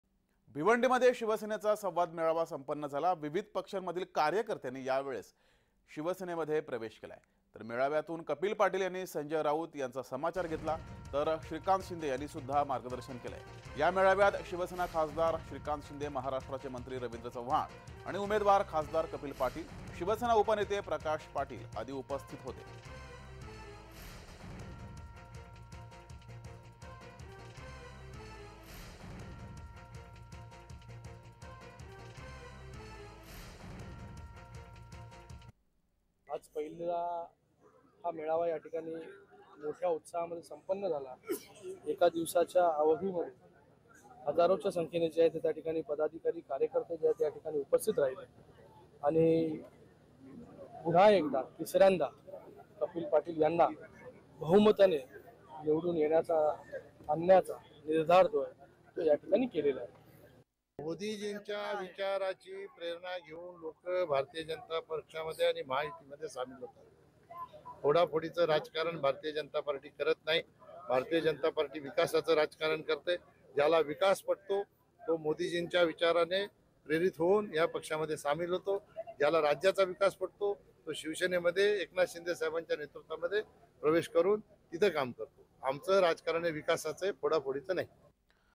Shivsena । भिवंडीत शिवसेनेचा संवाद मेळावा संपन्न । Marathi News
भिवंडीत शिवसेनेचा संवाद मेळावा संपन्न
विविध पक्षांमधील कार्यकर्त्यांचा शिवसेनेत प्रवेश